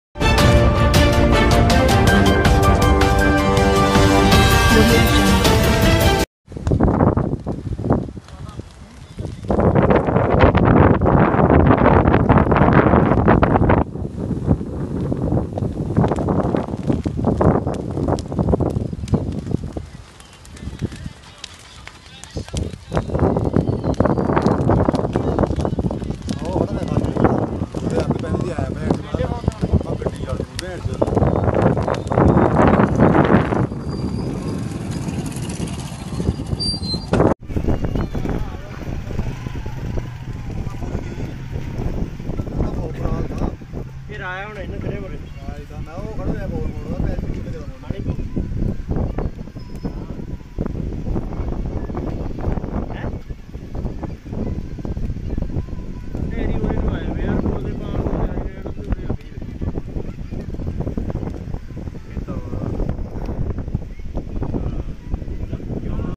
नालागढ़ के तहत पंचायत ढांग मेंकिसान की मोटर व मकान में लगीभयंकर आग मालिक की लापरवाही के कारण आग लगने
नालागढ़ के तहत पंचायत ढांग मेंकिसान की मोटर व मकान में लगीभयंकर आग  मालिक की लापरवाही के कारण आग लगने की बताई जा रही बजह